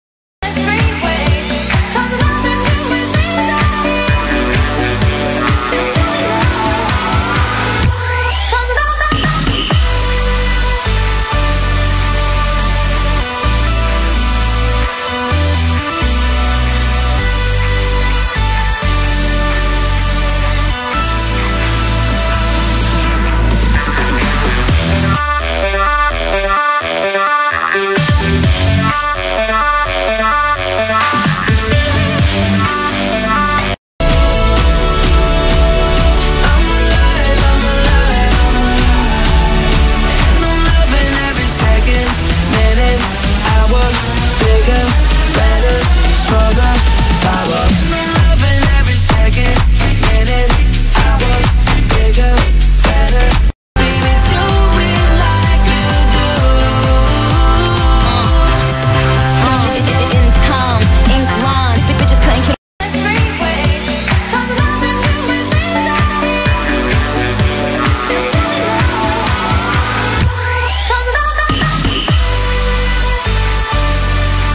Mes animaux
Mes animaux y enn a certain car j'ennai beaucoup y a des animaux qui sont morts :-(